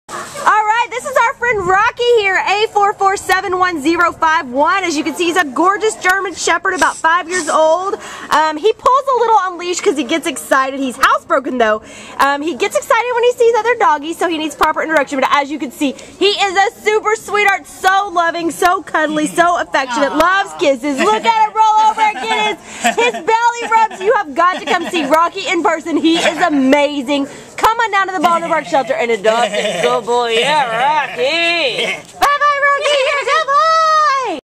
A4471051 Rocky | German shepherd
A4471051 Rocky is a gentlemanly 5-year-old neutered male German Shepherd whose former owner left him at the Baldwin Park Animal Care Center because they said they had allergies. Weighing 76 lbs, Rocky is a magnificent boy with a regal face that he is happy to have you kiss. He is affectionate, loyal and smart. He is playfully curious around other dogs, but does come on strong so could use some guidance in proper introduction as not all dogs will appreciate his greeting style. Sadly, he seems to have been neglected: his ears have fly strikes, which is evidence of living largely outdoors, and he has a somewhat odd gait that we think should be examined by an outside veterinarian. Still, it’s clear that Rocky has so much love in his heart,  he will be a devoted best friend in a loving home that can give him some initial TLC.  He will reward you with his loyalty and kisses forever.